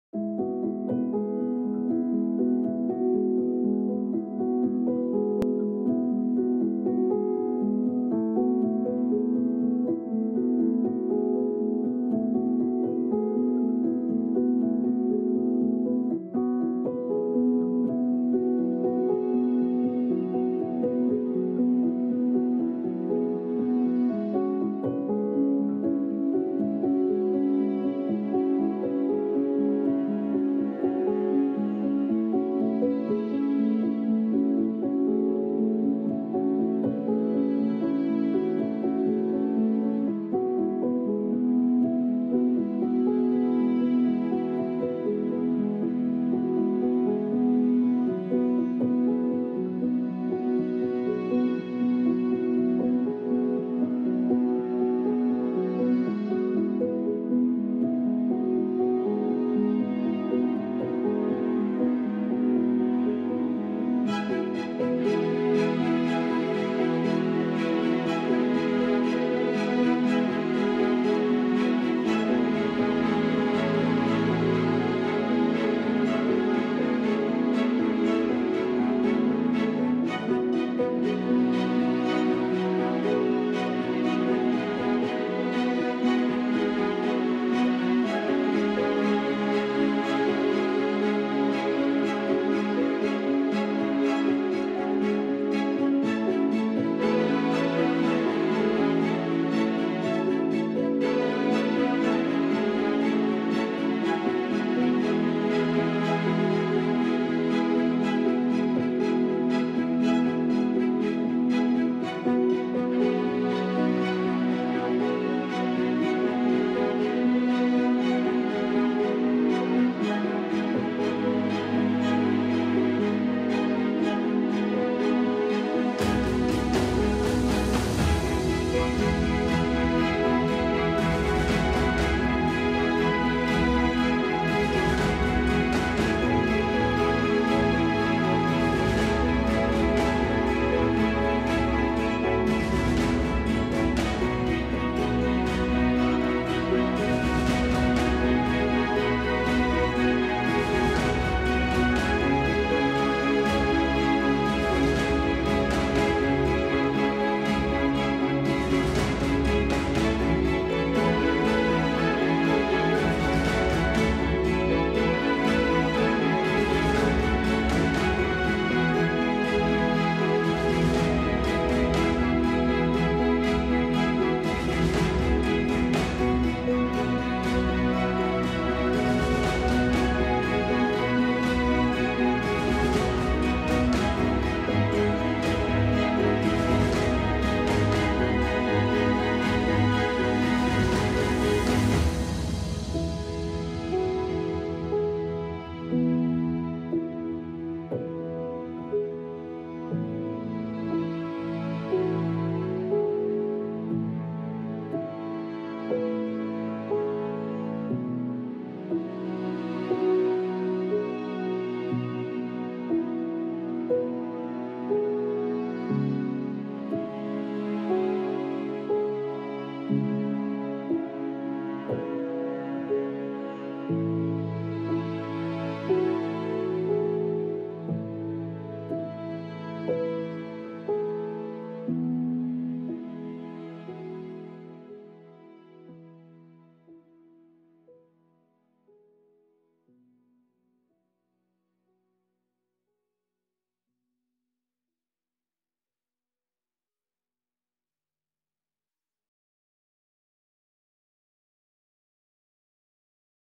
Springtime at Stoneleigh
Take a peek inside the colorful sights of springtime at Stoneleigh: a natural garden.